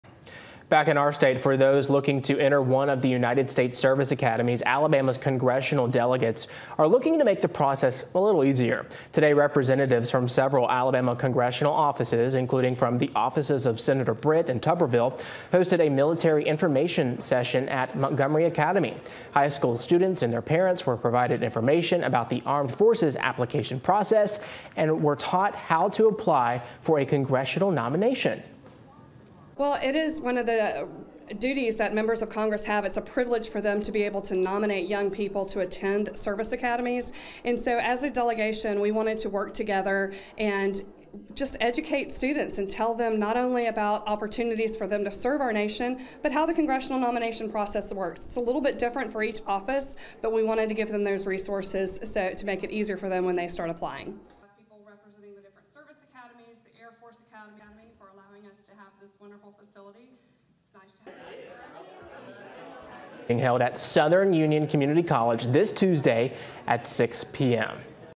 Alabama congressional offices host military information session
Representatives from several Alabama congressional offices hosted a military information session at Montgomery Academy.